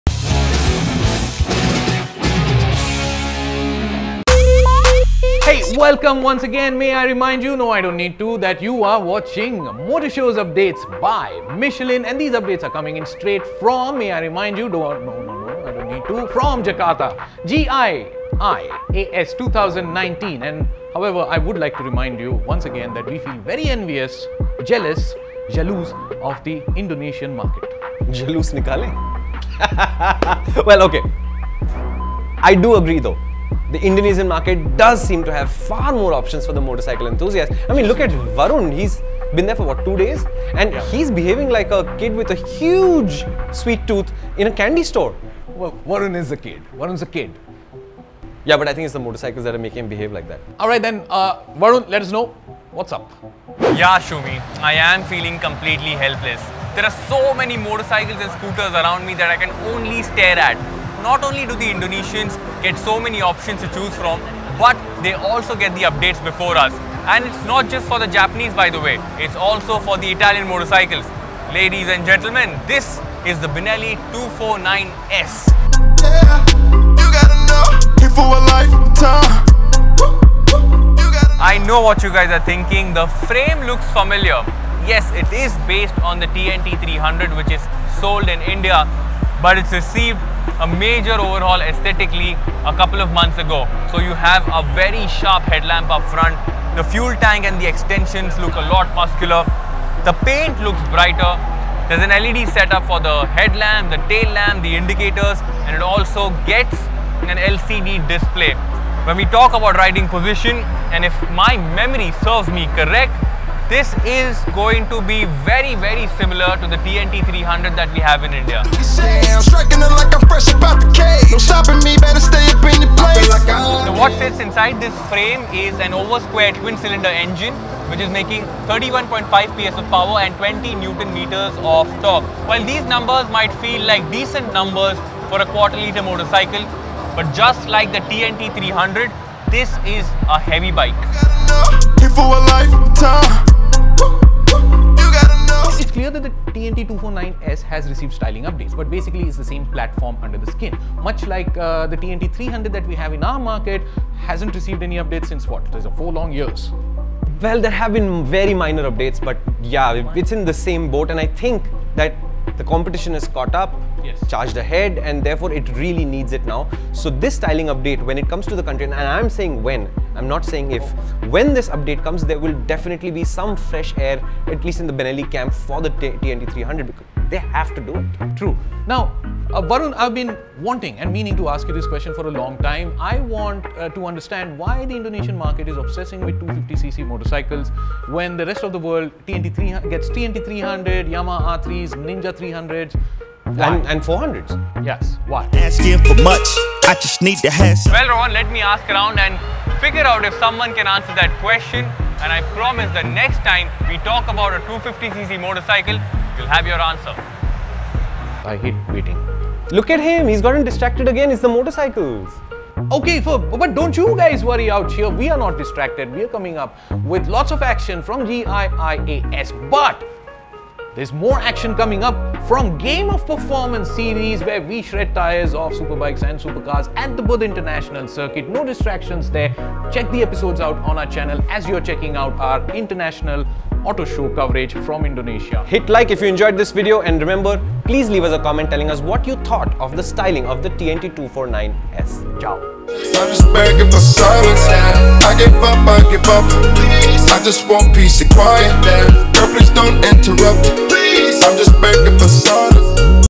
Benelli 249S : Time for an upgrade : GIIAS 2019 : PowerDrift
The Benelli 249S is the Italian brand's newest flavor. Packing a twin cylinder inside that familiar chassis, it has received quite a bit of aesthetic overhaul this time around. Varun Painter gets up close and personal with this baby Benelli at GIIAS 2019 while Rohan and Shumi continue their banter at the studio.